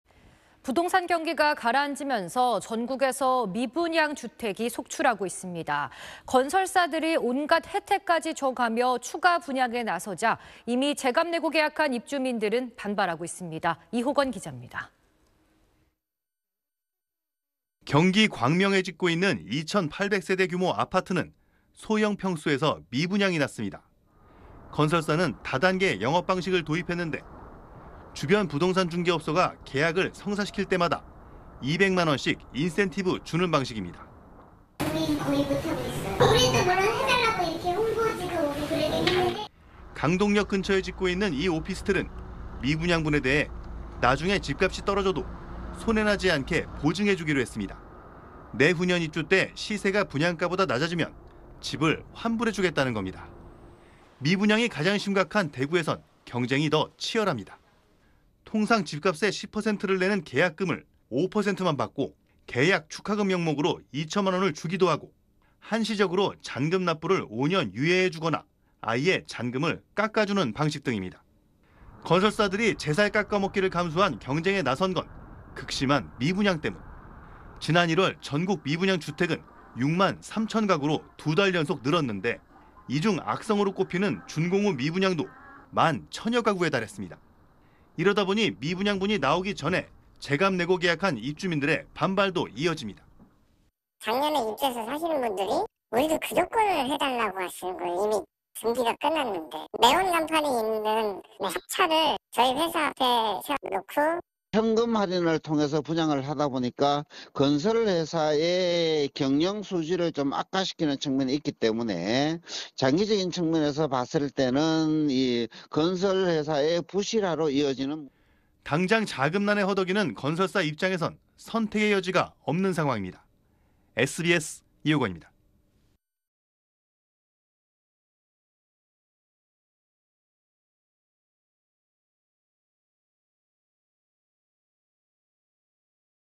다단계 영업에 계약 축하금…'미분양 털이' 안간힘 / SBS
〈앵커〉

부동산 경기기 가라앉으면서 전국에서 미분양 주택이 속출하고 있습니다. 건설사들이 온갖 혜택까지 줘가며 추가 분양에 나서자, 이미 제 값내고 계약한 입주민들은 반발하고 있습니다.

이호건 기자입니다.

〈기자〉

경기 광명에 짓고 있는 2천8백 세대 규모 아파트는 소형평수에서 미분양이 났습니다.

건설사는 다단계 영업방식을 도입했는데, 주변 부동산 중개업소가 계약을 성사시킬 때마다 2백만 원씩 인센티브 주는 방식입니다.

[근처 공인중개사 : 저희는 거의 못하고 있어요. 우리도 물론 해달라고 이렇게 홍보지도 오고 그러긴 했는데….]

강동역 근처에 짓고 있는 이 오피스텔은 미분양 분에 대해 나중에 집값이 떨어져도 손해 나지 않게 보증해주기로 했습니다.

내후년 입주 때 시세가 분양가보다 낮아지면 집을 환불해주겠다는 겁니다.

미분양이 가장 심각한 대구에선 경쟁이 더 치열합니다.

통상 집값의 10%를 내는 계약금을 5%만 받고 계약 축하금 명목으로 2천만 원을 주기도 하고, 한시적으로 잔금 납부를 5년 유예해주거나 아예 잔금을 깎아주는 방식 등입니다.

건설사들이 제살 깎아먹기를 감수한 경쟁에 나선 건 극심한 미분양 때문.

지난 1월 전국 미분양 주택은 6만 3천 가구로 두 달 연속 늘었는데, 이 중 악성으로 꼽히는 준공 후 미분양도 1만 1천여 가구에 달했습니다.

이러다 보니 미분양분이 나오기 전에 제값 내고 계약한 입주민들이 반발도 이어집니다.

[건설사 관계자 : 작년에 입주해서 사시는 분들이 우리도 그 조건 해달라고 하시는 거예요. 이미 등기가 끝났는데. 네온 간판이 있는 탑차를 회사 앞에 세워 놓고….]

[서진형/한국부동산경영학회 회장 : 현금 할인 통해 분양하다 보니까, 건설회사 경영 수지를 악화시키는 측면이 있기 때문에 장기적인 측면에서 봤을 때는 건설회사 부실화로 이어지는….]

당장 자금난에 허덕이는 건설사 입장에선 선택의 여지가 없는 상황입니다.

(영상편집 : 소지혜, 디자인 : 손승필, VJ : 박현우)